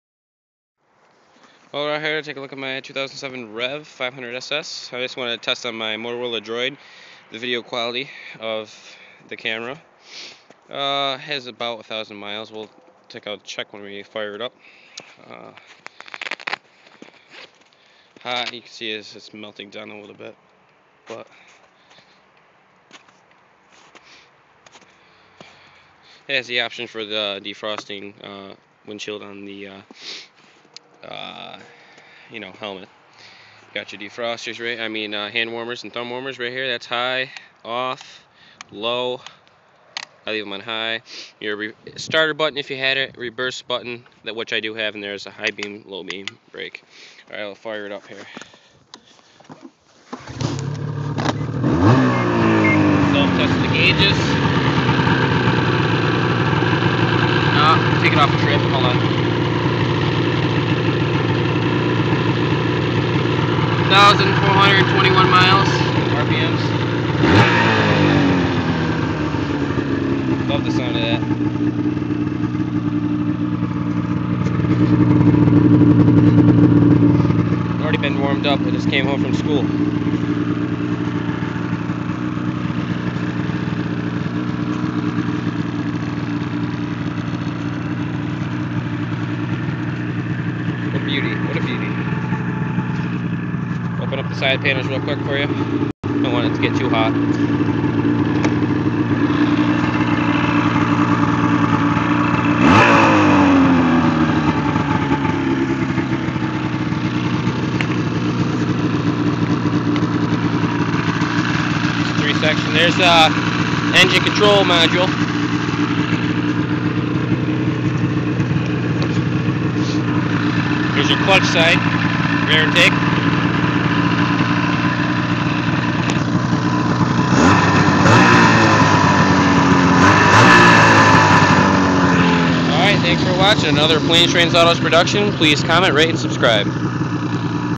Motorola Droid Test 2007 Ski-doo rev
This video was uploaded from an Android phone. A test of video quality from my Motorola Droid. 2007 Ski-Doo Mxz rev is shown. Hopefully I will have a video with editing on the sled soon.